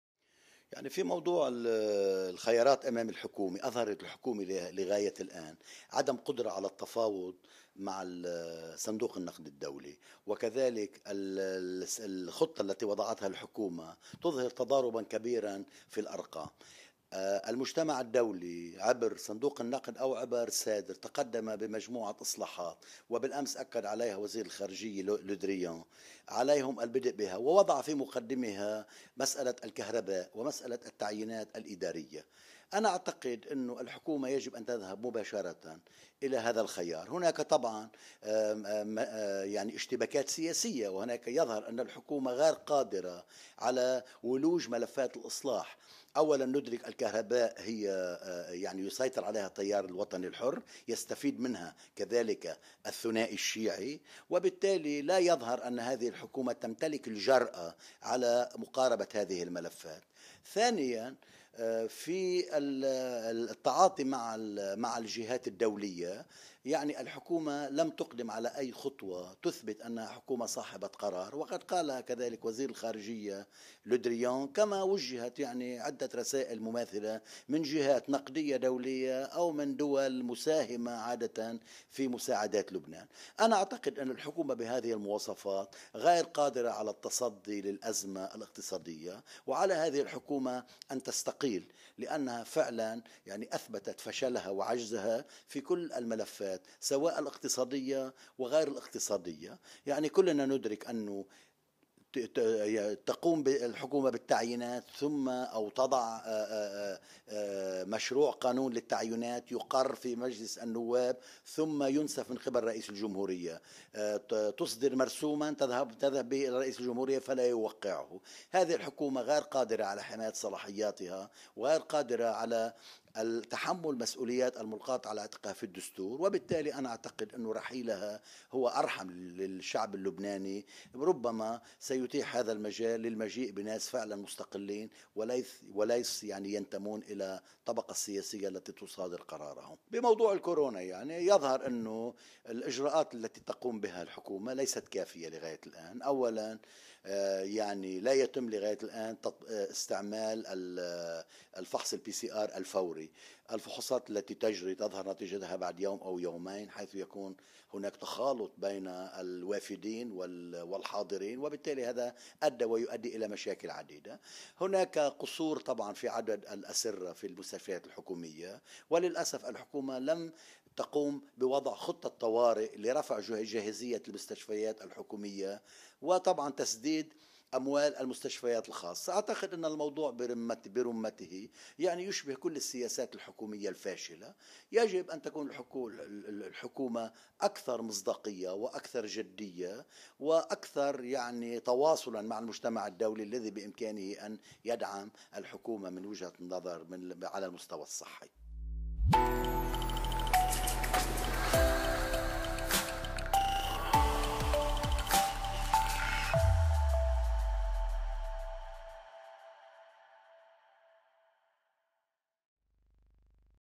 خالد حمادة ل"نيوزغيت": "على هذه الحكومة ان تستقيل لانها اثبتت فشلها وعجزها في كل الملفات".
اكد المحلل العسكري والاستراتيجي العميد خالد حمادة عبر موقعنا ان "اظهرت هذه الحكومة لغاية الان عدم قدرة على التفاوض مع صندوق النقد الدولي وكذلك الخطة التي وضعتها الحكومة تظهر تضاربا كبيرا في الارقام".

واضاف ان "المجتمع الدولي عبر الصندوق الدولي وعبر "سيدر" تقدم بمجموعة من الاصلاحات عليهم البدء بها كمسألة الكهرباء ومسألة التعيينات، ان هذه الحكومة غير قادرة على التصدي للازمة الاقتصادية و عليها ان تستقيل لانها اثبتت فشلها وعجزها في كل الملفات".